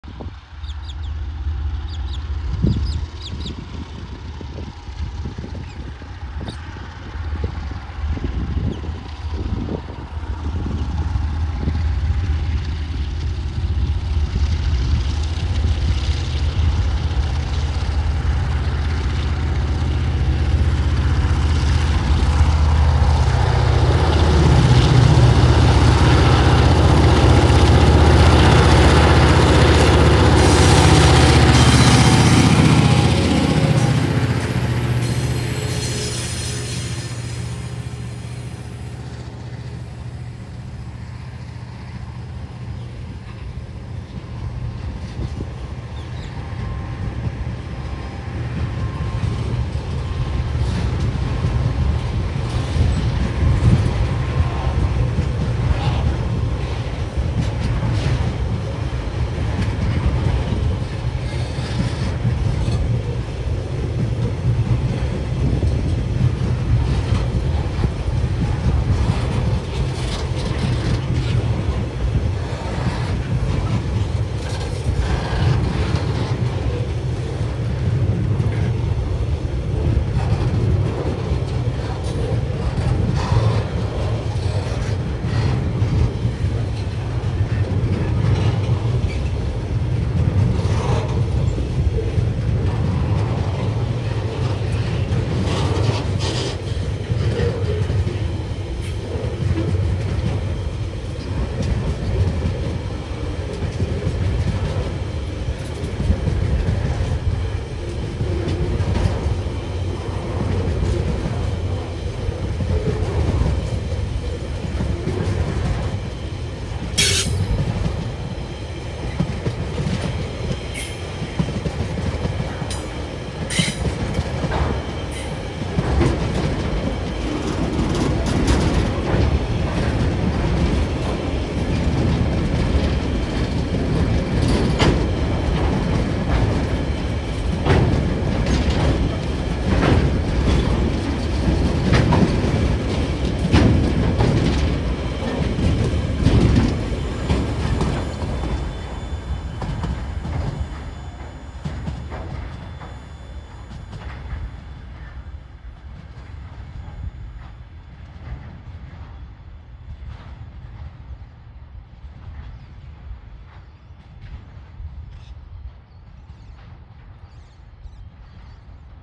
Tasrail 2054 TR04 #34 train Mayfield
TasRail 2054 leading train #534 powering at Mayfield on Friday the 9th of February 2024.